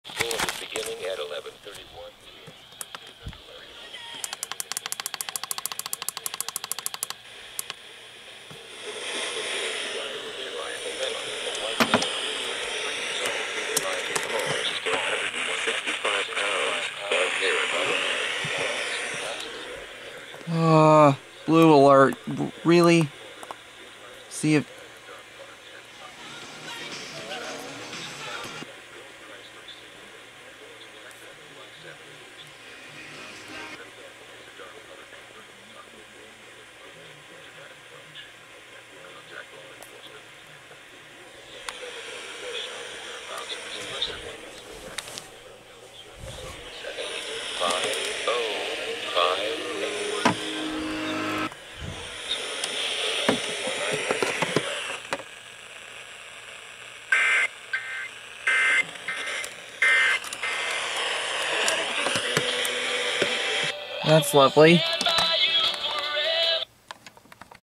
EAS #799-800: Florida Blue Alert on WJPT and WXKB || 2/22/22
--- Event Info
Event Type: Blue Alert (BLU)
Station(s): WXKB Cape Coral FL (103.9 MHz); WJPT Fort Myers FL (106.3 MHz)
Time: 11:31 PM EST
Counties Involved: All of Florida

A Florida BLUE Alert has been issued for Georgy Ryan Miedema, a white male, 33 years old, 5 feet 8 inches tall, 165 pounds, brown hair and blue eyes, last seen in the area of US 19 South Deer Run Road in Perry, Florida, who was last seen wearing a dark colored tank top. He may be traveling in a 2000, gold Chrysler Sebring, FL tag number Y78TKU. The vehicle has a dark colored convertible top.
If located, DO NOT APPROACH. Contact law enforcement immediately. If you have any information on the whereabouts of this person please contact the Taylor County Sheriff's Office at or 911.